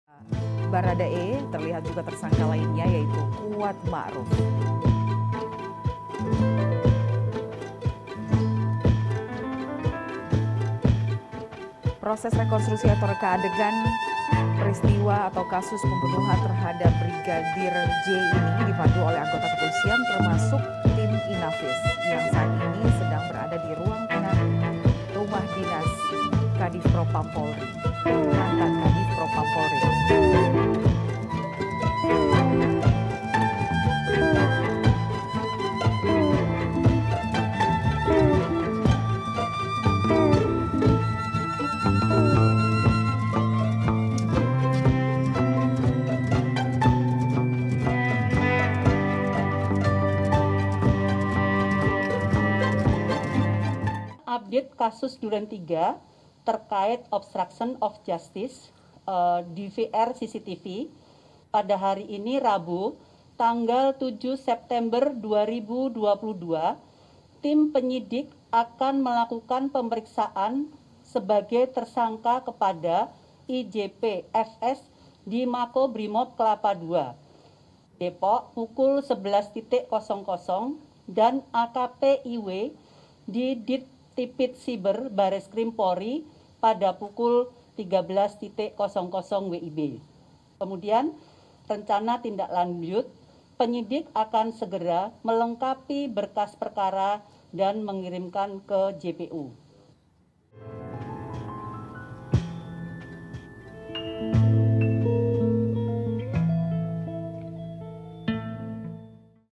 Ferdy Sambo Kembali Diperiksa Kasus Obstruction of Justice
Tersangka perkara obstruction of justice atau upaya menghalang halangi proses hukum, Ferdy Sambo, Rabu pagi kembali menjalani pemeriksaan terkait dugaan perusakan DVR CCTV di Duren Tiga.